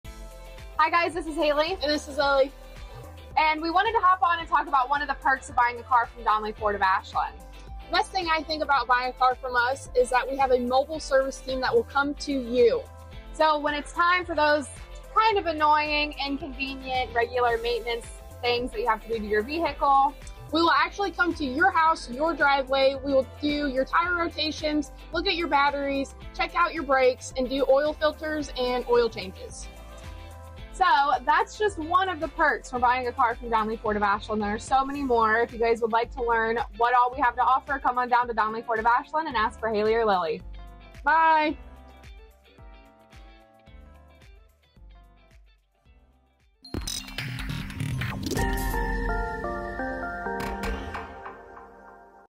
🌟PART 2🌟One of the best perks of buying a vehicle from Donley Ford...is our Ford Mobile Service Van!
🌟🌟PART 2🌟🌟

One of the best perks of buying a vehicle from Donley Ford...is our Ford Mobile Service Van!! When you need minor/routine maintenance just give our mobile service number a call and we will get you on the books!!